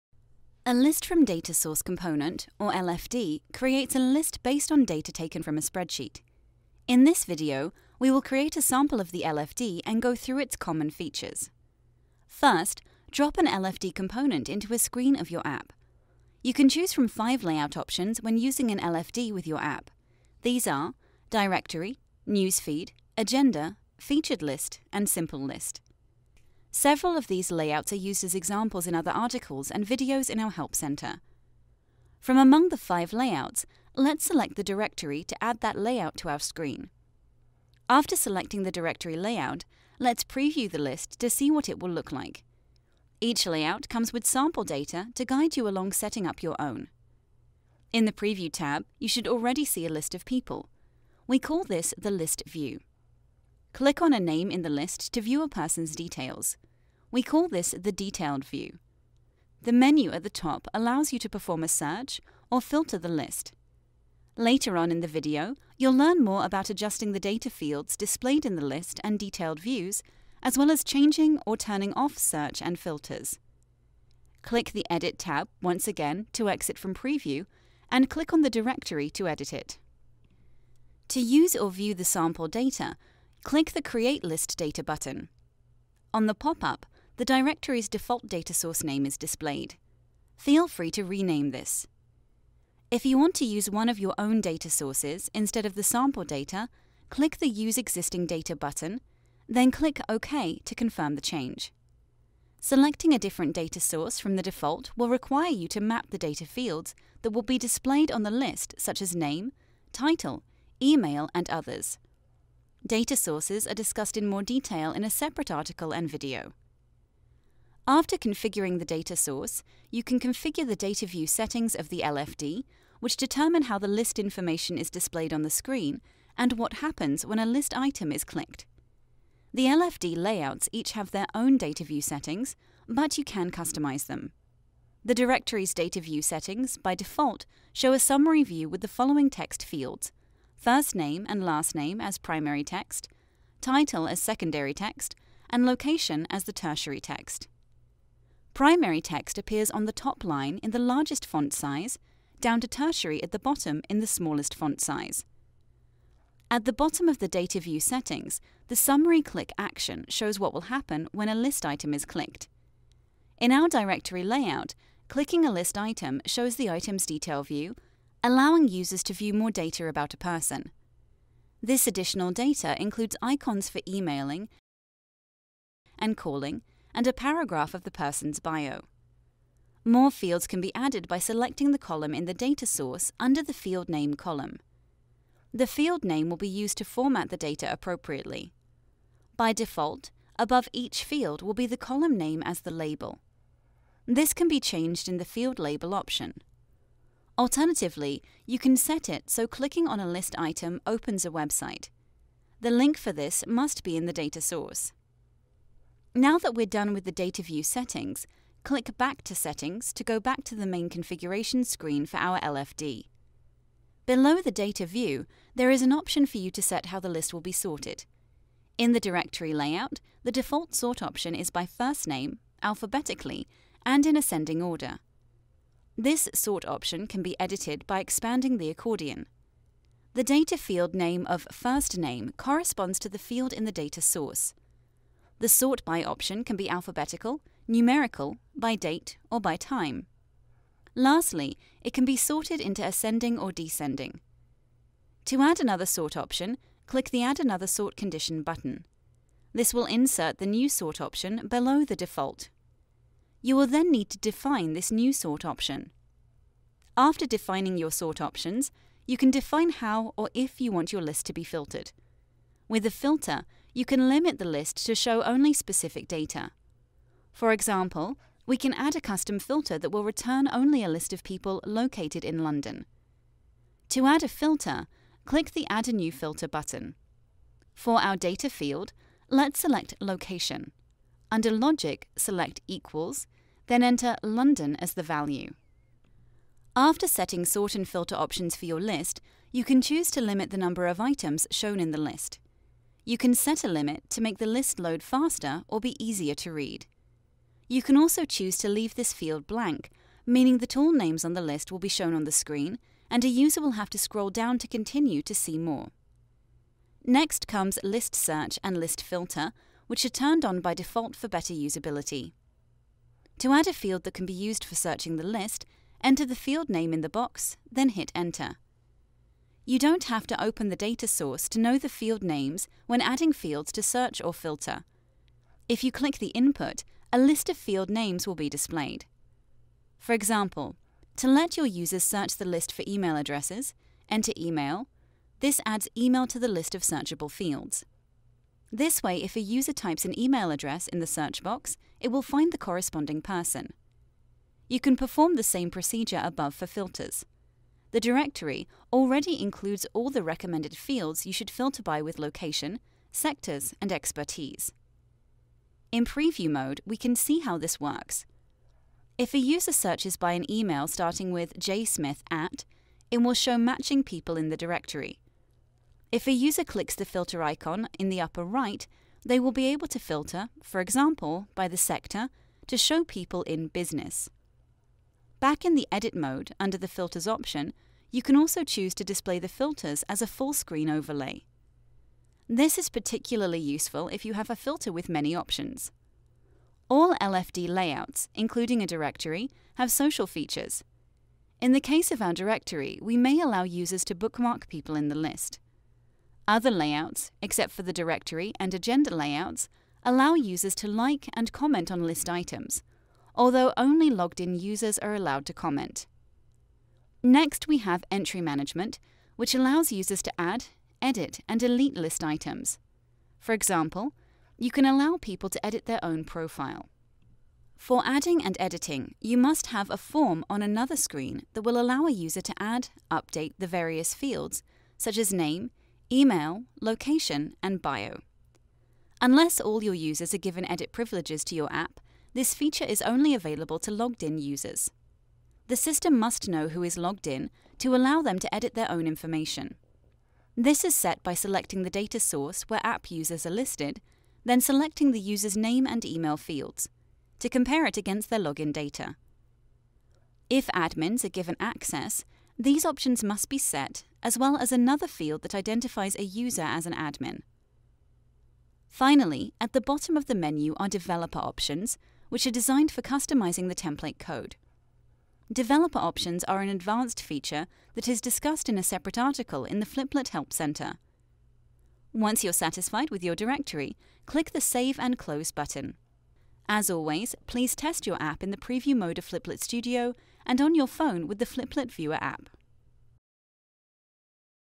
List from Data Source Component
A video on how to use the List from Data Source (LFD) Component in Fliplet Studio